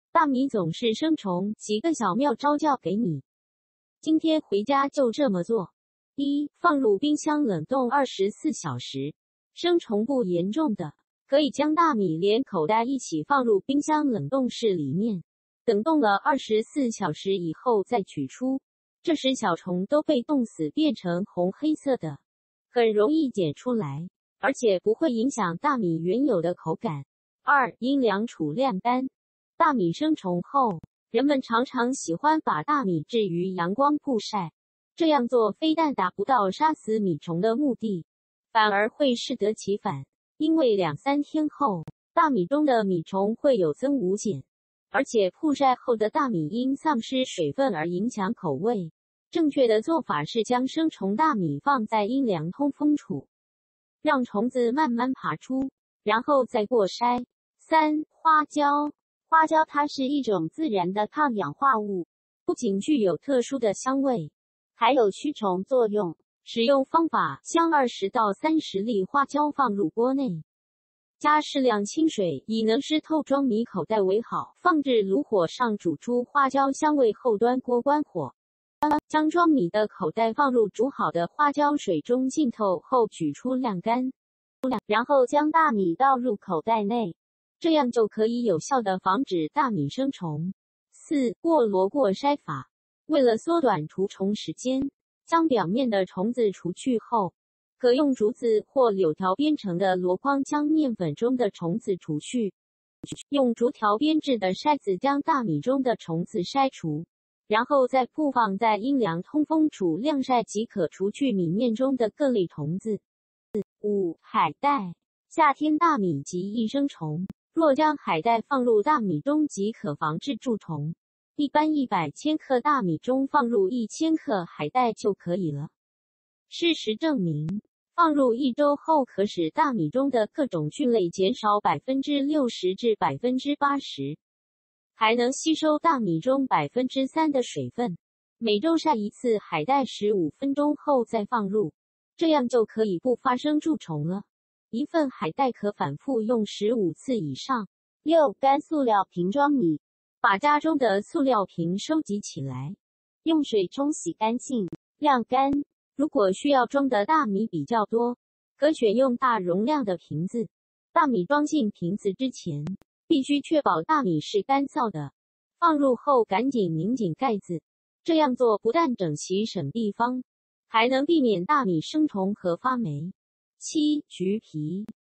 大米總是生蟲？教你７個方法預防大米生蟲
大米總是生蟲？幾個小妙招教給你，今天回家就這麼做！
1.放入冰箱冷凍24小時
生蟲不嚴重的，可以將大米連口袋一起放入冰箱冷凍室裡面，等凍了24小時以後再取出，這時小蟲都被凍死變成紅黑色的，很容易撿出來，而且不會影響大米原有的口感。
2.陰涼處晾乾
大米生蟲後，人們常常喜歡把大米置於陽光曝曬，這樣做非但達不到殺死米蟲的目的，反而會適得其反，因為兩三天後，大米中的米蟲會有增無減，而且曝曬後的大米因喪失水分而影響口味。
正確的做法是將生蟲大米放在陰涼通風處，讓蟲子慢慢爬出，然後再過篩。
3.花椒
花椒它是一種自然的抗氧化物，不僅具有特殊的香味，還有驅蟲作用。
使用方法：將20-30粒花椒放入鍋內，加適量清水（以能濕透裝米口袋為好）放置爐火上煮出花椒香味後端鍋關火，將裝米的口袋放入煮好的花椒水中浸透後取出晾乾，然後將大米倒入口袋內。這樣就可以有效的防止大米生蟲。
4.過籮過篩法
為了縮短除蟲時間，將表面的蟲子除去後，可用竹子或柳條編成的籮筐將麵粉中的蟲子除去，用竹條編制的篩子將大米中的蟲子篩除，然後再鋪放在陰涼通風處晾曬即可除去米麵中的各類蟲子。
5.海帶
夏天大米極易生蟲，若將海帶放入大米中即可防治蛀蟲。一般100千克大米中放入1千克海帶就可以了。
事實證明，放入一周後可使大米中的各種菌類減少60%至80%，還能吸收大米中3%的水分，每周曬一次海帶15分鐘後再放入，這樣就可以不發生蛀蟲了。一份海帶可反覆用15次以上。
6.乾塑料瓶裝米
把家中的塑料瓶收集起來，用水沖洗乾淨，晾乾。如果需要裝的大米比較多，可選用大容量的瓶子。大米裝進瓶子之前，必須確保大米是乾燥的，放入後趕緊擰緊蓋子。
這樣做不但整齊省地方，還能避免大米生蟲和發黴。
7.橘皮
橘皮中含有豐富的檸檬醛，曬乾後放入大米中，不僅可以防蛀殺蟲，大米中還會有一種橘皮的清香。